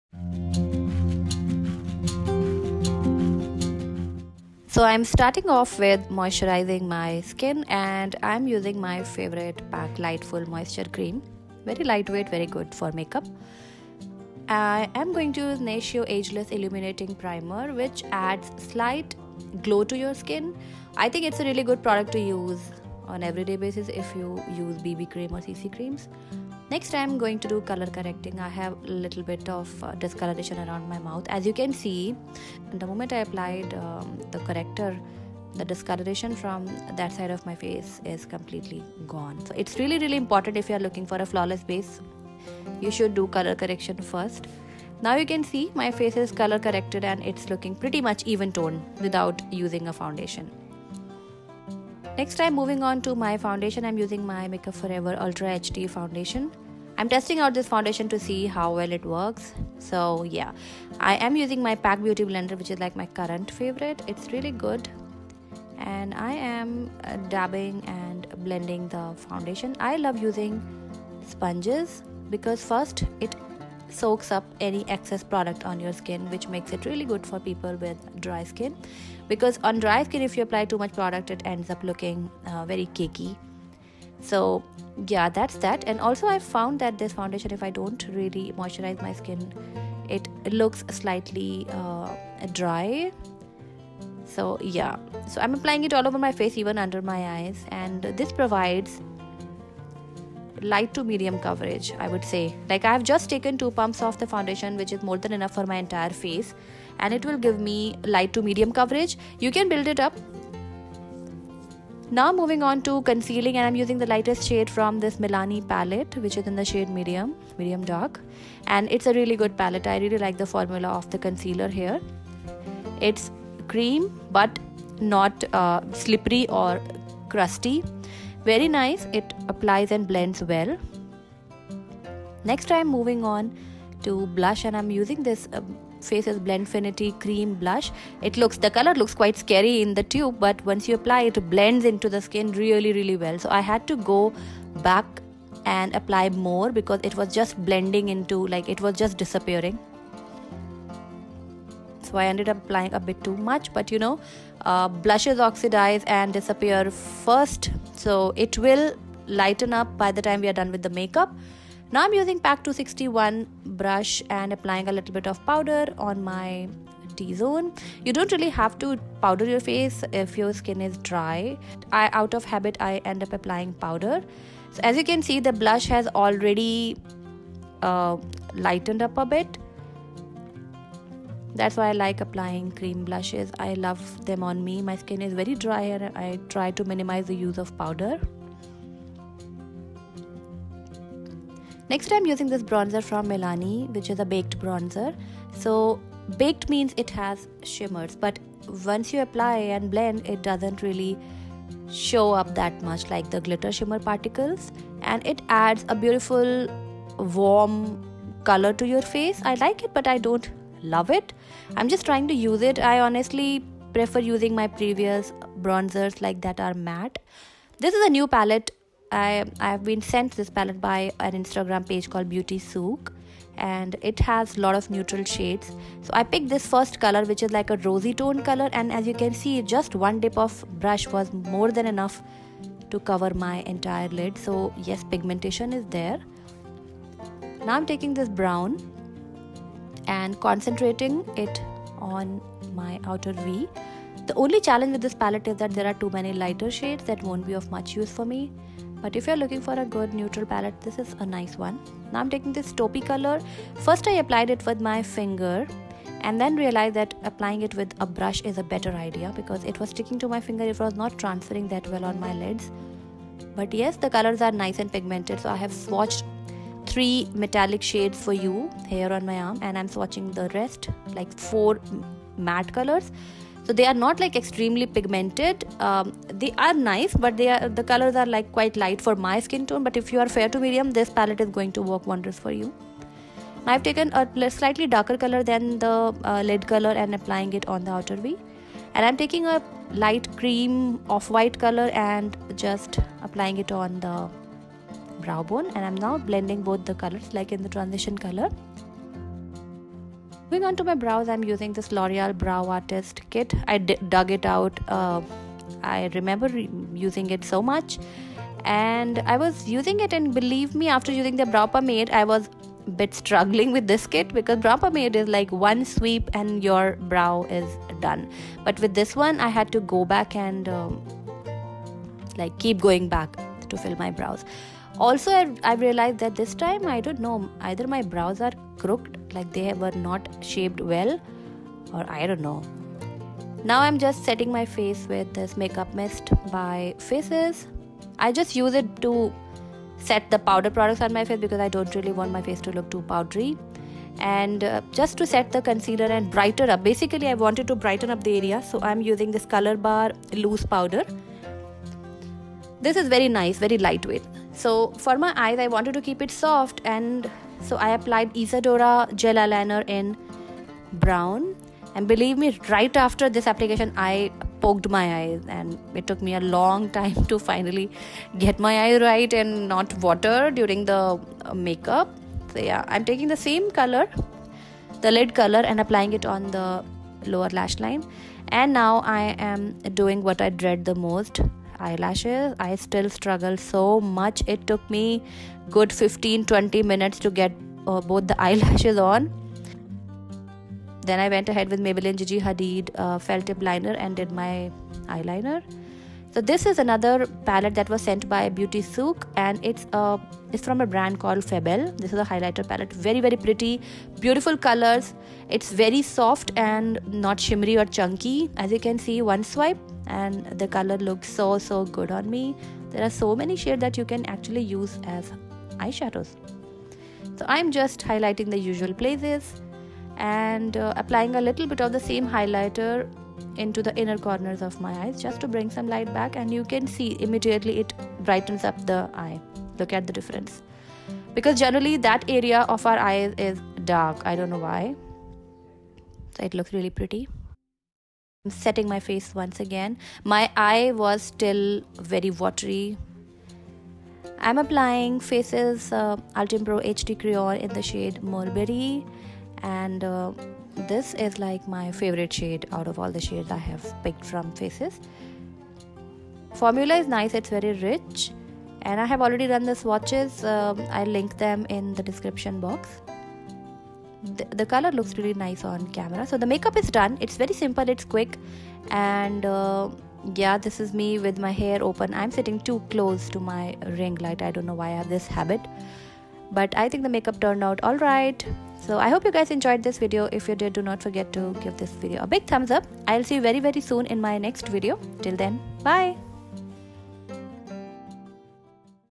Easy Makeup - Winged Eye liner + Bold Lips
Products Used -

PAC lightful Moisturiser
Natio Ageless Illuminating Lotion
Make Up For Ever HD Foundation
Milani Conceal + Perfect All In One Correcting Kit
Faces Ultime Pro Blend Finity Stick Blush - Stunning Cinnamon 03
Kiko Baked Powder 04
Milani Baked Bronzer - 05 Soleil

Maybelline GIGI hadid Felt Tip Eyeliner

Loreal Genius Brow Artist Kit - Medium

Faces UltimPro HD Lip Crayon - Mulberry Magic